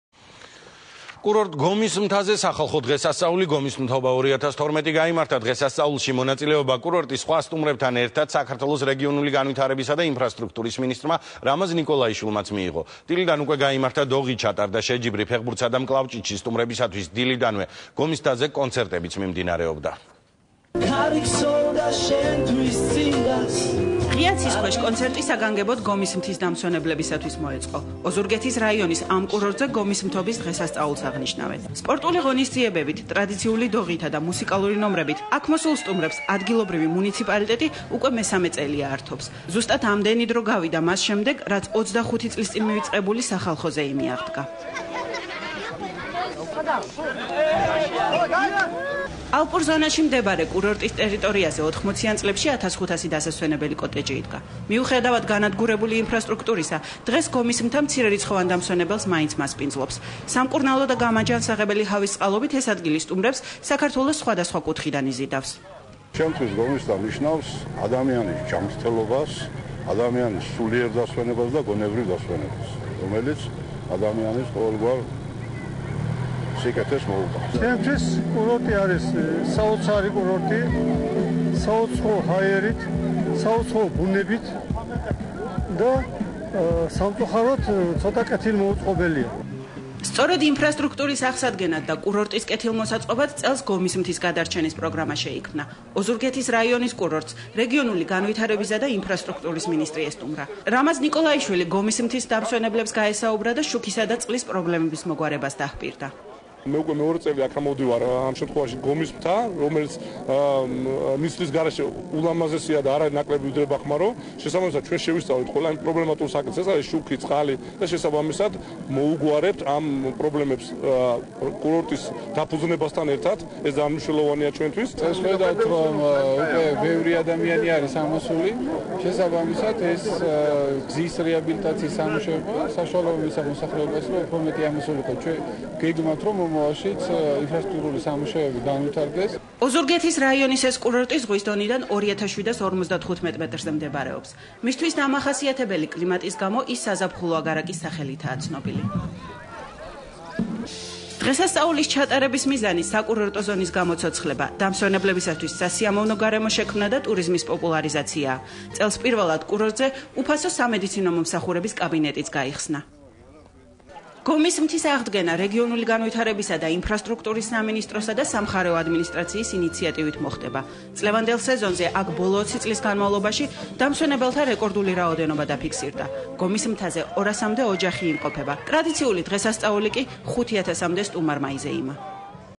ნიკოლაიშვილი გომისმთობაზე
კურორტ გომისმთაზე,  დღეს, სახალხო დღესასწაული "გომისმთობა-2012" გაიმართა. დღესასწაულში მონაწილეობა, კურორტის სხვა სტუმრებთან ერთად, საქართველოს რეგიონული განვითარებისა და ინფრასტრუქტურის მინისტრმა რამაზ ნიკოლაიშვილმა მიიღო. დღესასწაულის ფარგლებში მოეწყო დოღი, ჩატარდა შეჯიბრება ფეხბურთსა და მკლავჭიდში, გაიმართა ოზურგეთის მუნიციპალური კულტურის ცენტრითან არსებული ანსამბლ "ლურჯი იალქანისა" და ჩოხატაურის კულტურის ცენტრთან არსებული ანსამბლის "გურული ბიჭების" კონცერტი. ღონისძიებაში მონაწილეობა ადგილობრივმა შემსრულებლებმაც მიიღეს.

კურორტი გომისმთა საქართველოს ერთ-ერთი ყველაზე მაღალმთიანი კურორტია, რომელიც მდინარე ბჟუჟის ხეობაში, ზღვის დონიდან 2350 მეტრის სიმაღლეზე მდებარეობს. გომისმთის კლიმატი თავისი სამკურნალო თვისებებითაა ცნობილი. კურორტი გარშემორტყმულია წიწვოვანი ტყით და ბუნებრივი წყაროებით. გომისმთასთან ახლოსაა ჭინჭაოს ტბა, რომელიც ზღვის დონიდან 2500 მეტრზე მდებარეობს.  უკვე ტრადიციად ქცეული დღესასწაული "გომისმთობა",  კურორტზე მესამე წელია იმართება. დღესასწაულის მიზანი საკურორტო ზონის გამოცოცხლება, დამსვენებლებისათვის სასიამოვნო გარემოს შექმნა, ტურისტების მოზიდვა  და ტურიზმის პოპულარიზაციაა.